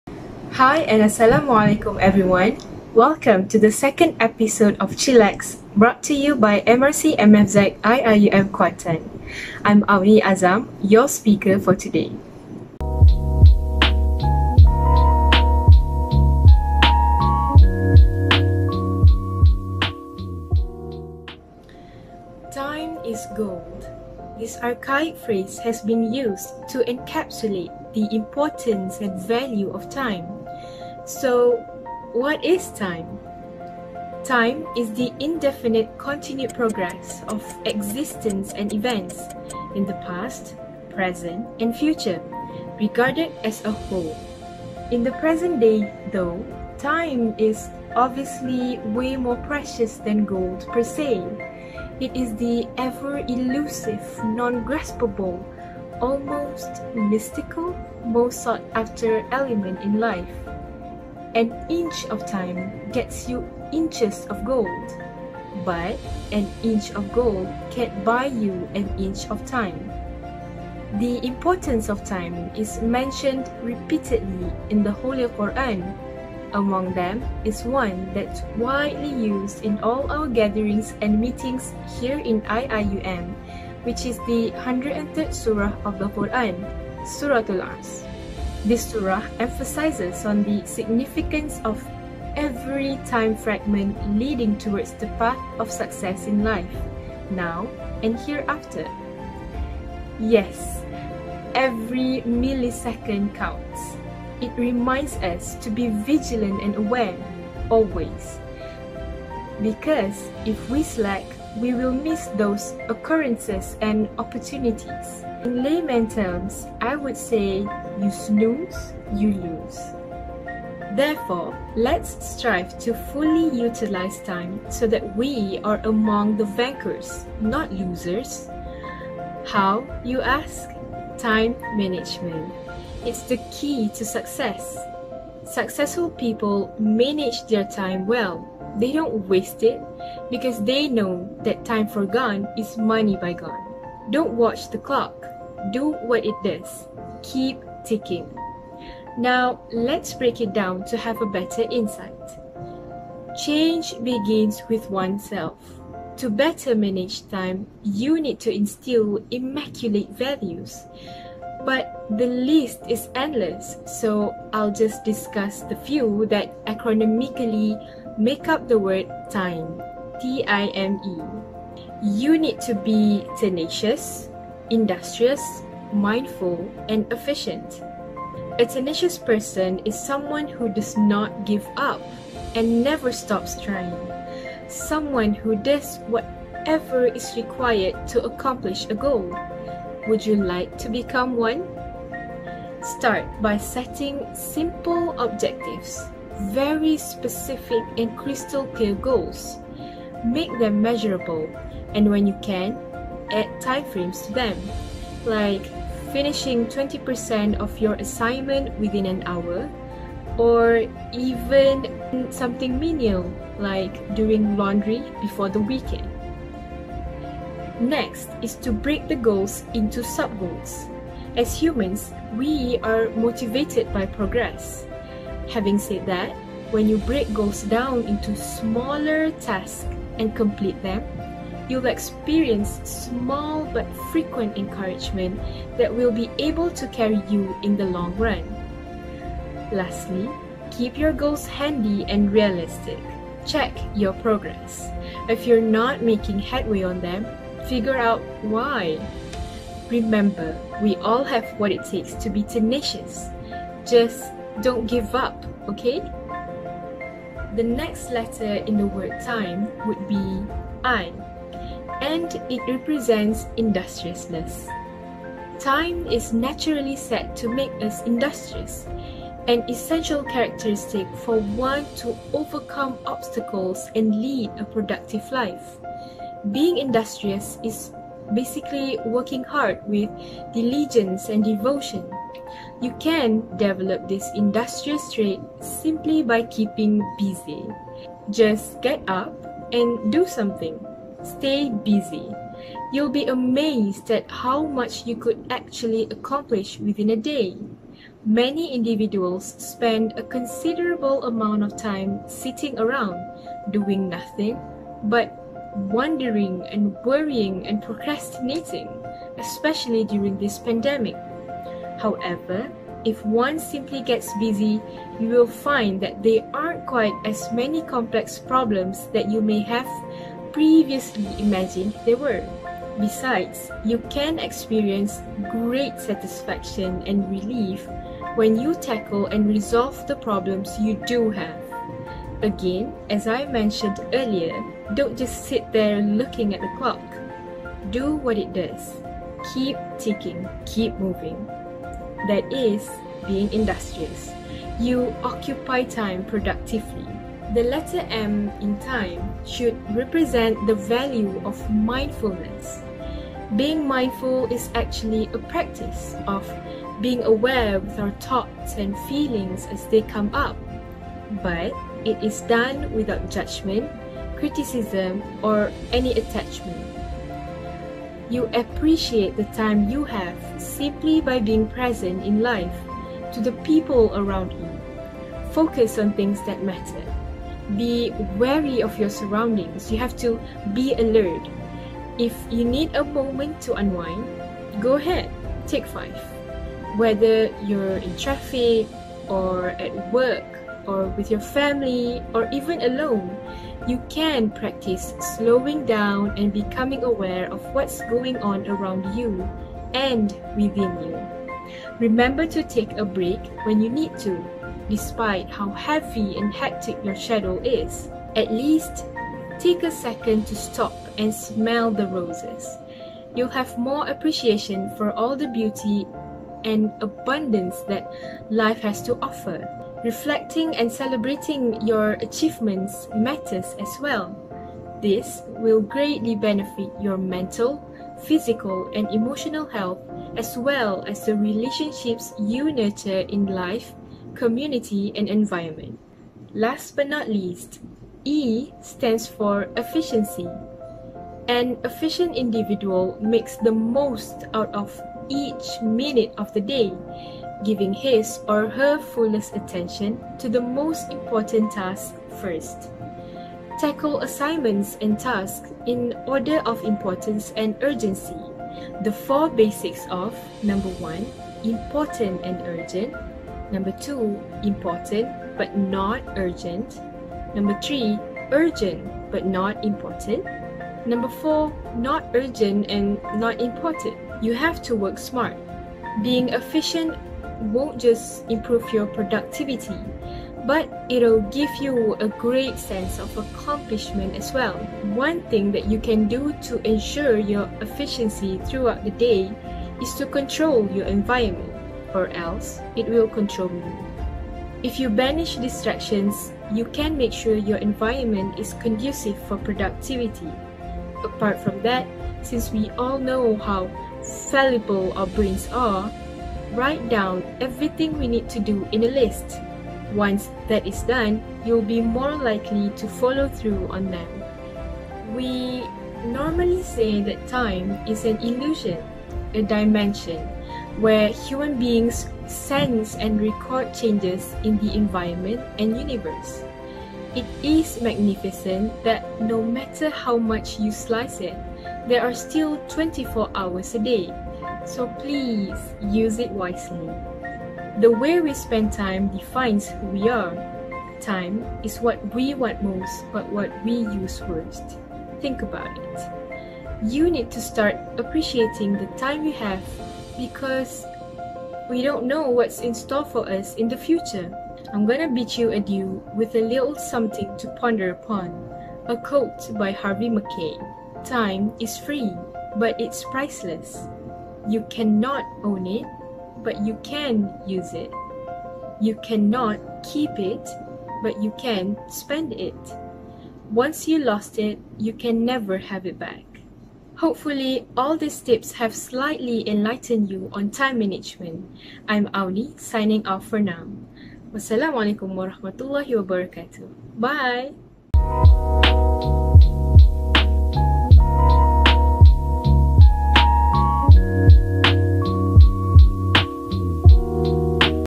Chillax 1.0 - Episode 2: Time Is Gold
🎙️[CHILLAX 1.0 EPISODE 2]🎙️

Dear all IIUM students,

CHILLAX 1.0, the long awaited series has come out with it second episode! 🤯

Do you still struggle to be productive in your daily life? Or you just wasting all the time with video games and drama. And you feel bad about yourself all the time because it leave you empty with less meaning in your life.

With an appealing topic entitled "Time is Gold", Sister Aunee would be sharing on how to manage your time efficiently as well as improving your productivity in your daily life. Let's watch it now !

Thank you, wassalam. ✨

Moving forward hand in hand.

Regards,
MFZ Training & Education Bureau,
MFZ Media and Publication Bureau.